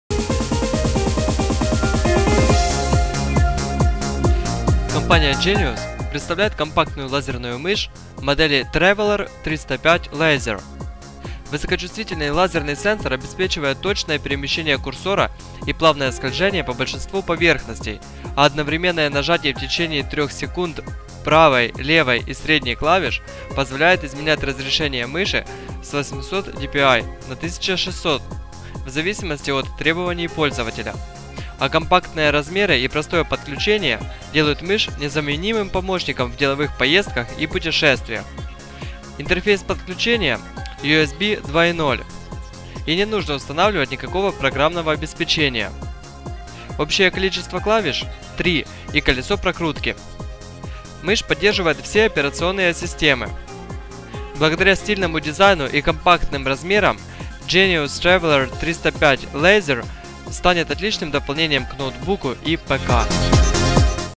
Genius Traveler 305 Laser обзор / review
Обзор проводной оптической мыши Genius Traveler 305 Laser
Genius Traveler 305 Laser wired optical mice review